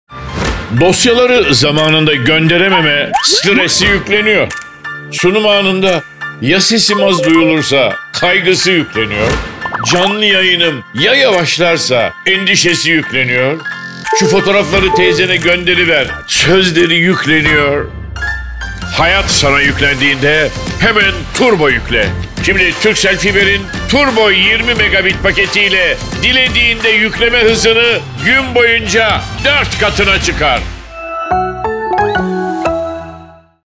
Turbo’yla yükleme hızını artır
Hayat sana yüklendiğinde Turkcell Fiber Turbo 20Mbps ile yükleme hızını gün boyu 4’e katla!